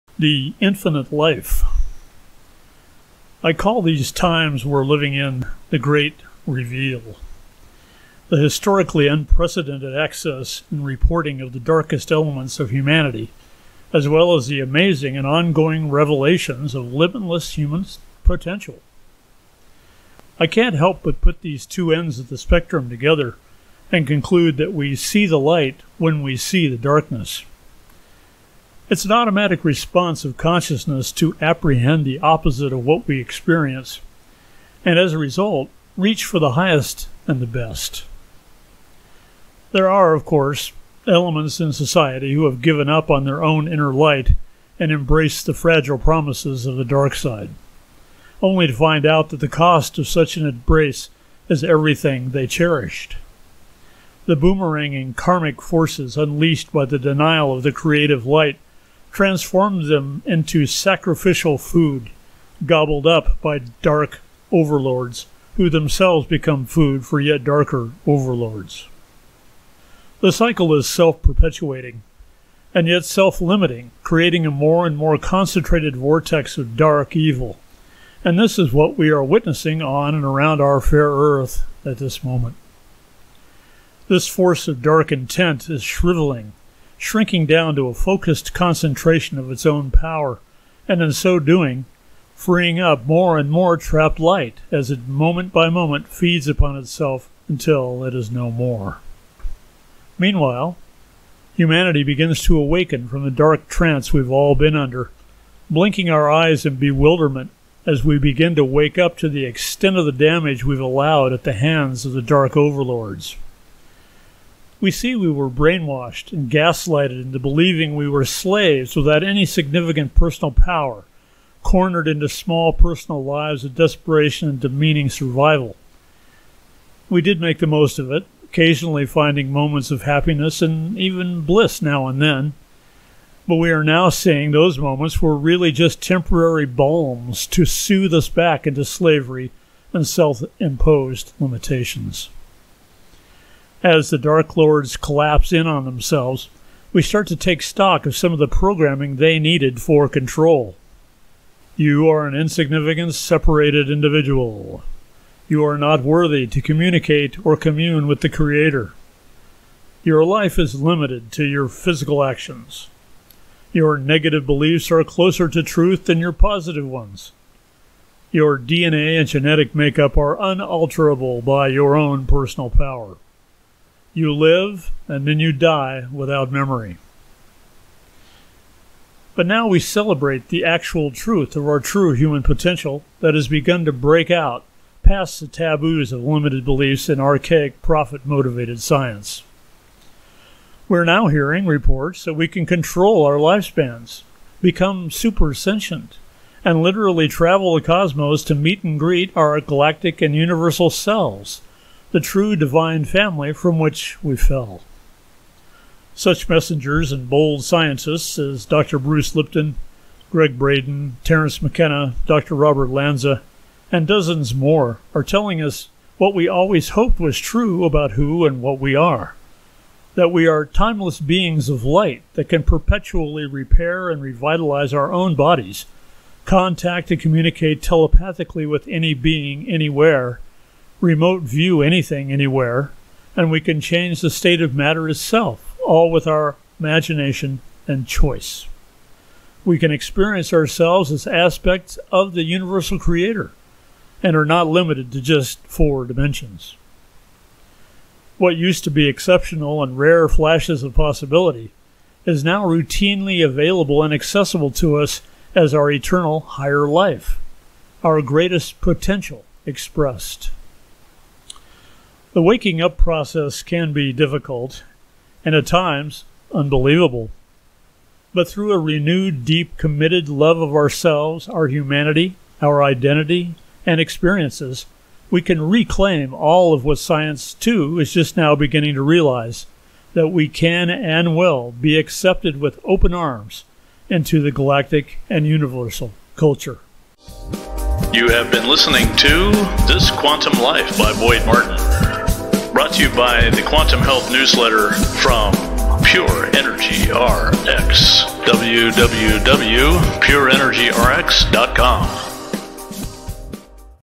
This Quantum Life #89 - The Infinite Life
I call these times we're living in, "The Great Reveal"--the historically unprecedented access and reporting of the darkest elements of humanity, as well as the amazing and ongoing revelations of limitless human potential. I can't help but put these two ends of the spectrum together and conclude that we see the light when we see the darkness.